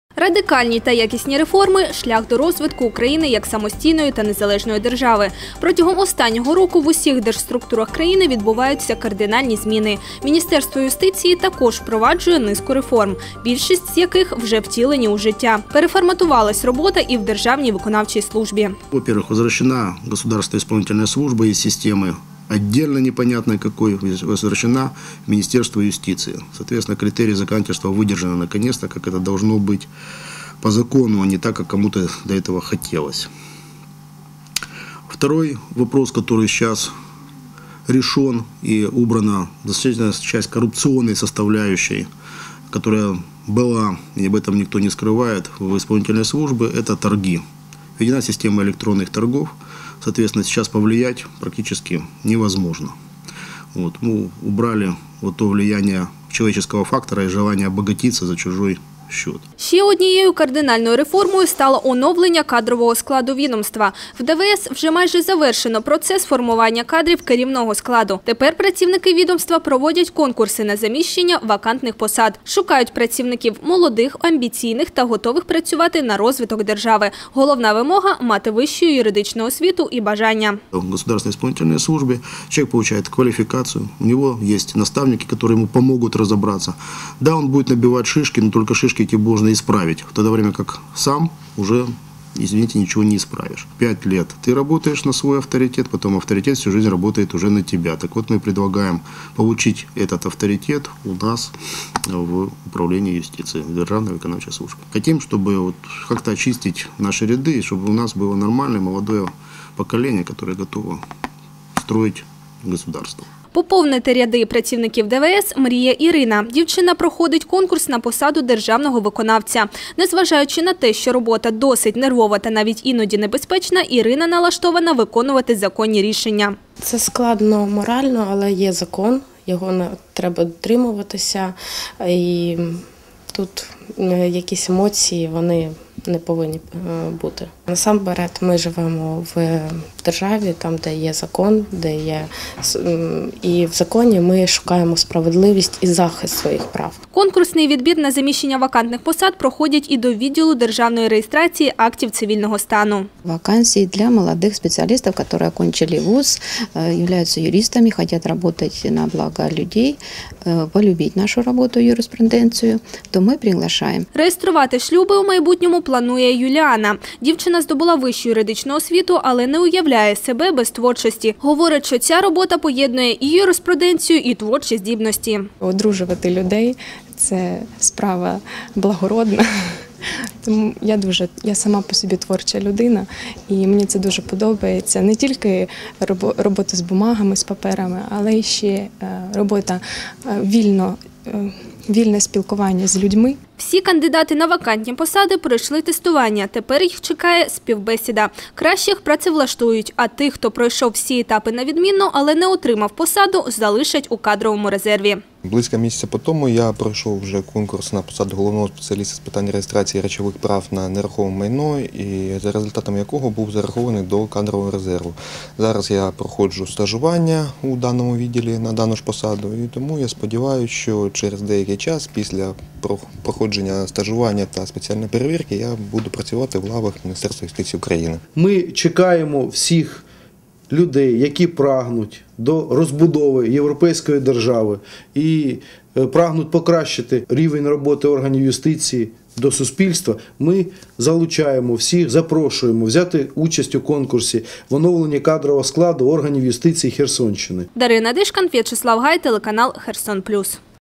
Юстиція запрошує на роботу молодих та амбіційних працівників
Радикальні та якісні реформи – шлях до розвитку України, як самостійної та незалежної держави. Протягом останнього року в усіх держструктурах країни відбуваються кардинальні зміни. Міністерство юстиції також впроваджує низку реформ, більшість з яких вже втілені у життя.  Переформатувалась робота і в державній виконавчій службі.
Ще однією кардинальною реформою стало оновлення кадрового складу відомства. В ДВС вже має завершено процес формування кадрів керівного складу. Тепер працівники відомства проводять конкурси на заміщення вакантних посад. Шукають працівників молодих, амбіційних та готових працювати на розвиток держави. Головна вимога – мати вищу юридичну освіту і бажання.